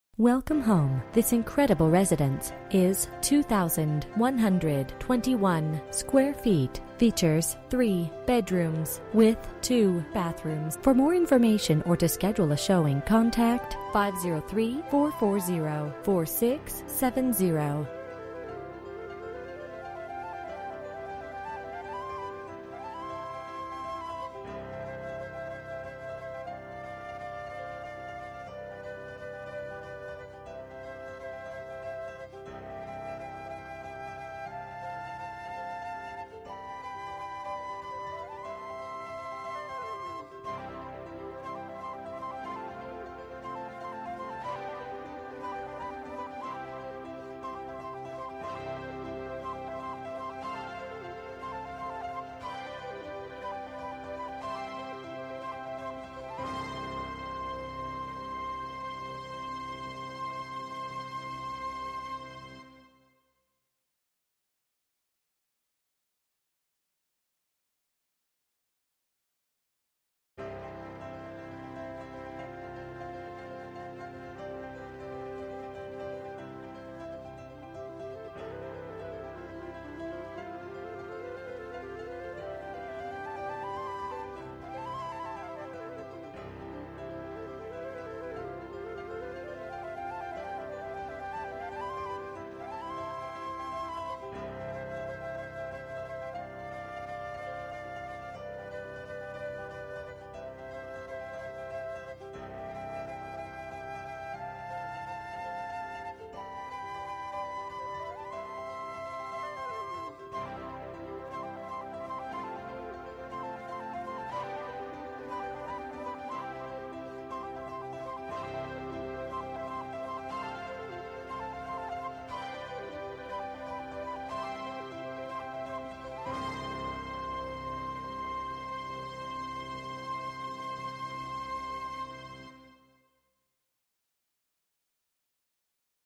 Homes for sale - 90009 Manion Dr, Warrenton, OR 97146
Beautifully remodeled single level home in the coveted Surf Pines! Move in ready, lots of upgrades including all brand new windows, hardwood floors throughout,  Nest smart thermostat, carbon monoxide, smoke alarms, fresh paint in & out!  Enjoy the brand new kitchen with quartz counter tops, high end cabinets with soft close drawers, tiled back splash, tiled floor plus a beautiful island that's ready for cooking while your family gathers around to talk about their day, not to mention the wet bar complete with a wine cooler! The slider off the kitchen leads out to your fenced back yard. Close to the beach and ready for the perfect family!

Bedrooms: 3
Bathrooms: 2
Square Feet: 2121
Price: $435,000